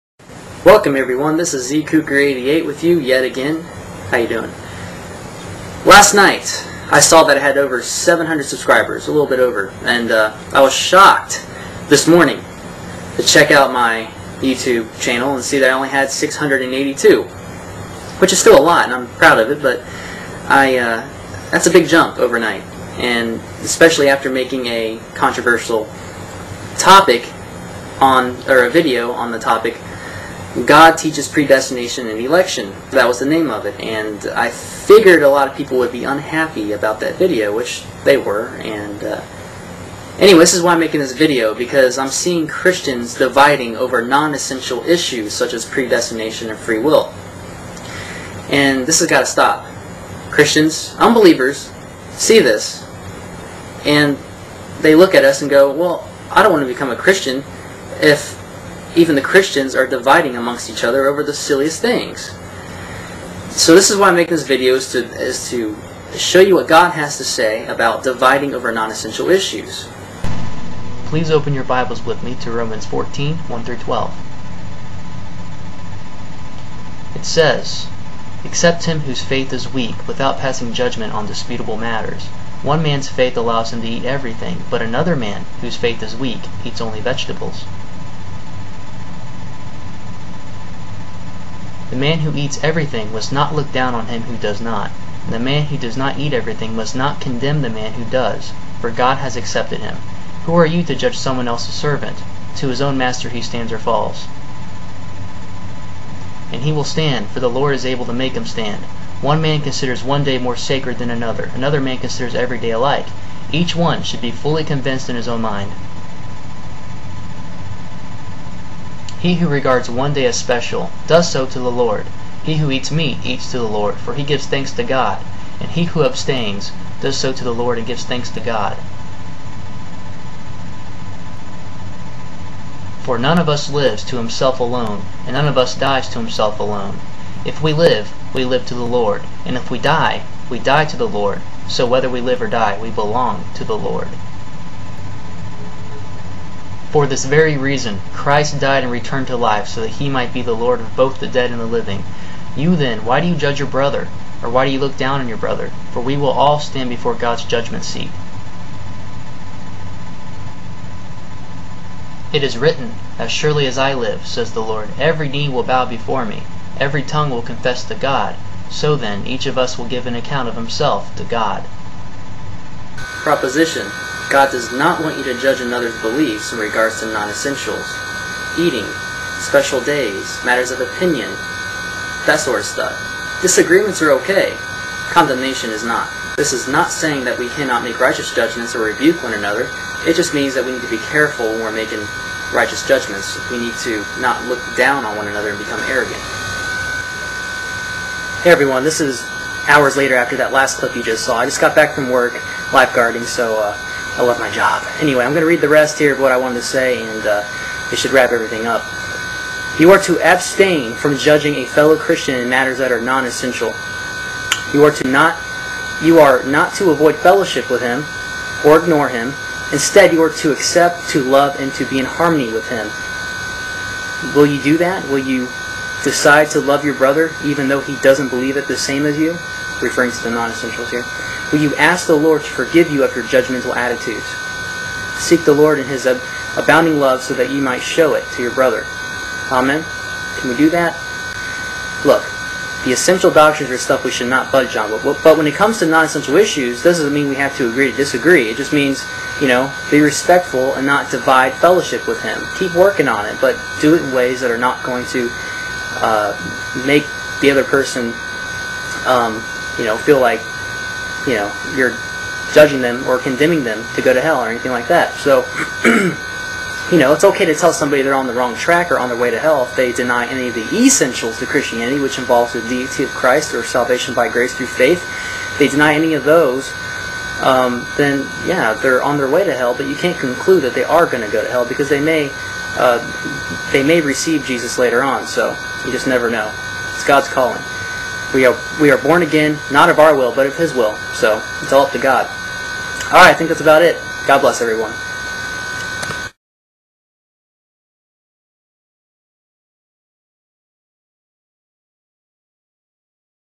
Don't Pass Judgment (zkueker88)
Romans 14:1-12

Accept him whose faith is weak, without passing judgment on disputable matters. 2One man's faith allows him to eat everything, but another man, whose faith is weak, eats only vegetables. 3The man who eats everything must not look down on him who does not, and the man who does not eat everything must not condemn the man who does, for God has accepted him. 4Who are you to judge someone else's servant? To his own master he stands or falls. And he will stand, for the Lord is able to make him stand. 5One man considers one day more sacred than another; another man considers every day alike. Each one should be fully convinced in his own mind. 6He who regards one day as special, does so to the Lord. He who eats meat, eats to the Lord, for he gives thanks to God; and he who abstains, does so to the Lord and gives thanks to God. 7For none of us lives to himself alone and none of us dies to himself alone. 8If we live, we live to the Lord; and if we die, we die to the Lord. So, whether we live or die, we belong to the Lord. 9For this very reason, Christ died and returned to life so that he might be the Lord of both the dead and the living. 10You, then, why do you judge your brother? Or why do you look down on your brother? For we will all stand before God's judgment seat. 11It is written: "`As surely as I live,' says the Lord, `every knee will bow before me; every tongue will confess to God.'" 12So then, each of us will give an account of himself to God.

Proposition: God doesn't want you to judge another's beliefs in regard to non-essentials, i.e. eating, special days, matters of opinion.